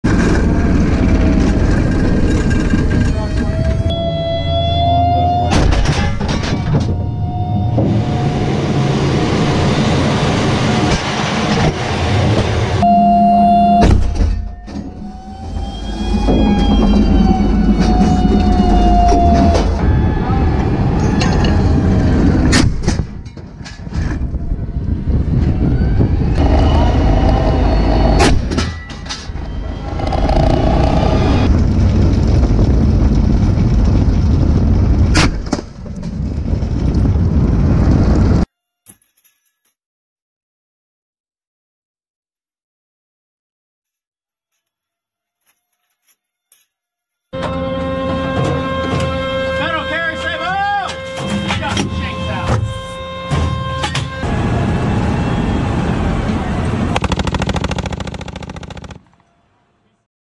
M1A1 "Abrams" firing range - crew camera shows loading mechanism
M1A1 Abrams main battle tank crew loading rounds during a live fire exercise. M1A1 is armed with a 120 mm L/44 M256A1 smoothbore gun, carrying 40 rounds. Footage is from marine training conducted in Kuwait's Camp Arifjan

In M1A1, ammunition is stored in directly opposite of the barrel loading mechanism and is loaded manually